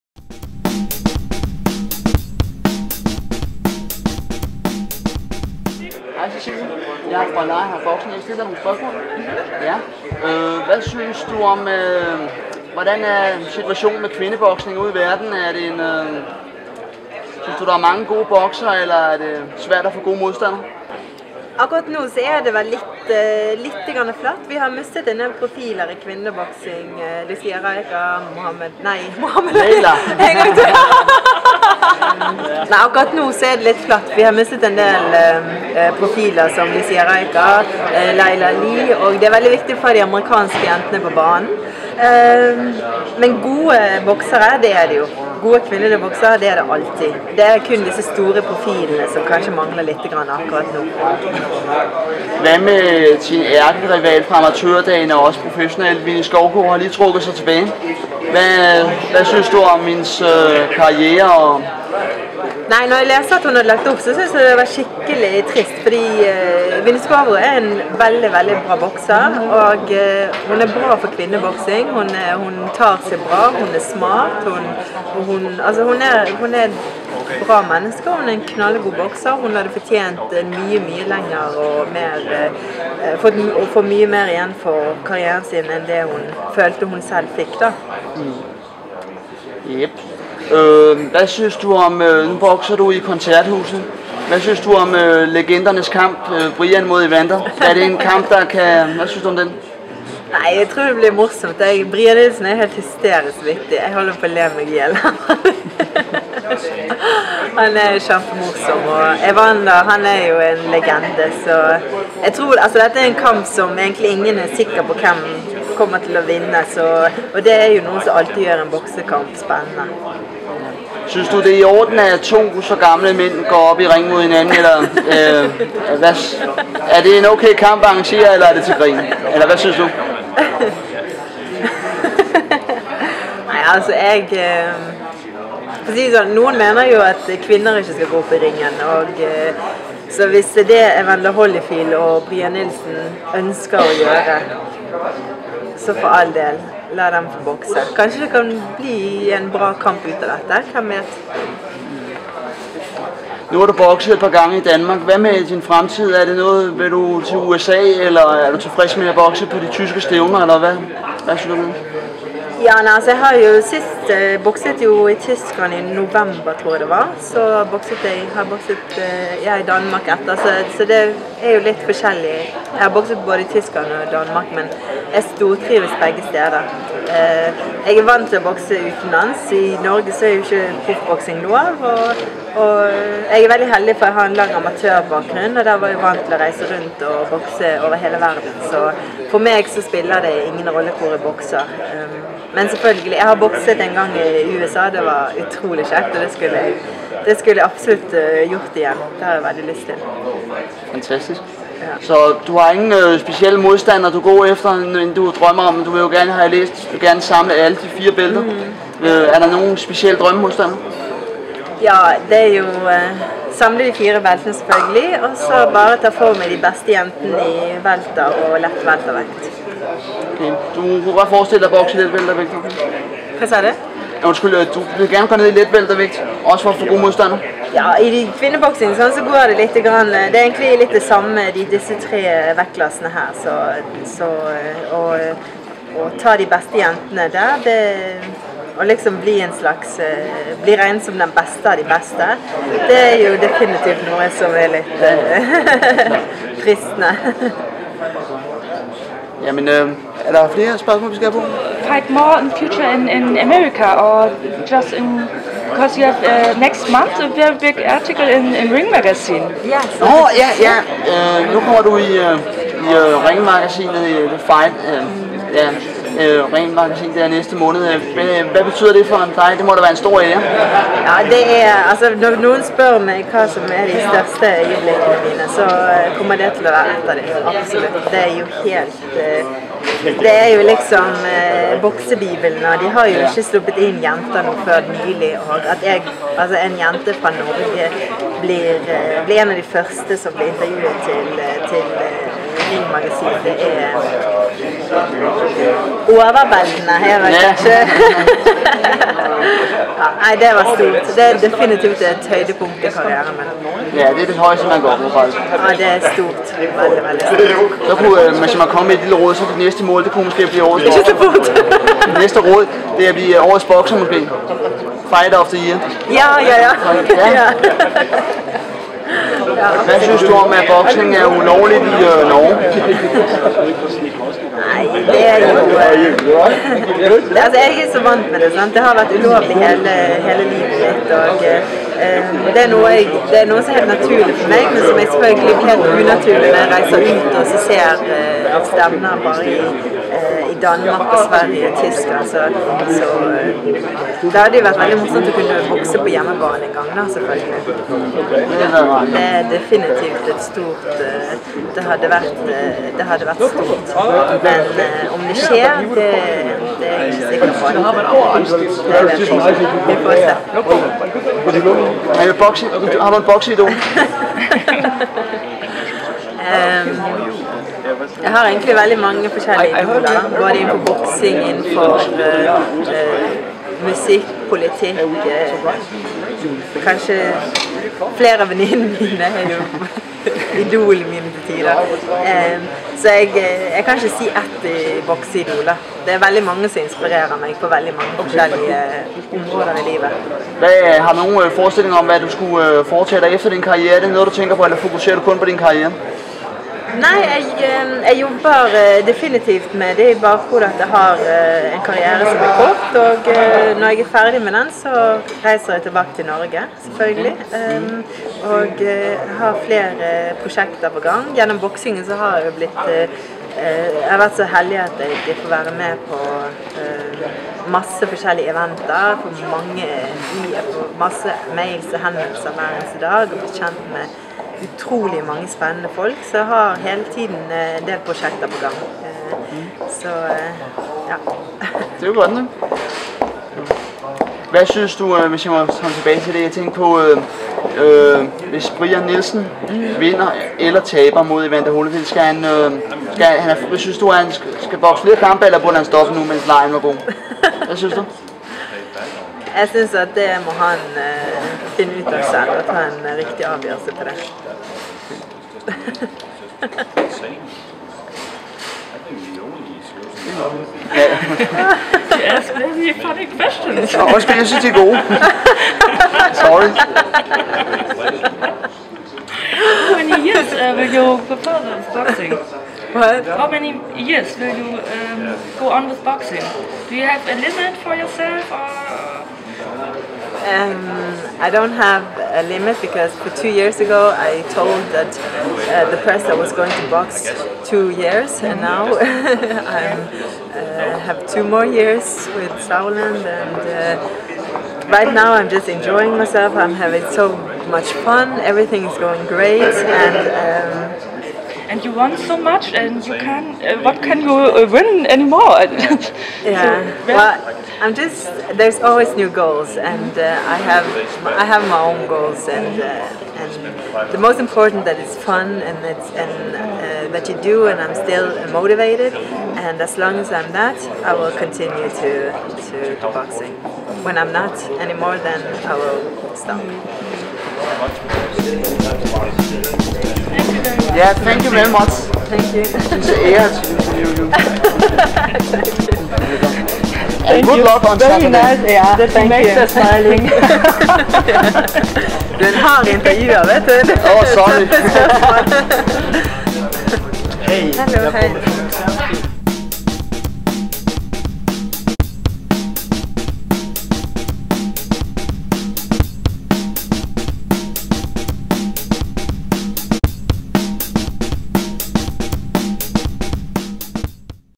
Cecilia Brækhus "FirstLady" in interview with Karsten Harbo Hansen.mov
Interview between 3 belts worldtitle holder female boxer "FirstLady" Cecilia Brækhus and Denmarks største boksning fan Karsten Harbo Hansen from Lionheart.Boxing in the Crowne Plaza Copenhagen Towers Hotel at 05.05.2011 before the "Legends of the Ring" evening in DR-Koncerthuset
Video- and photoproduction: BeateBodenhagen
Translation: KarstenHarboHansen, BeateBodenhagen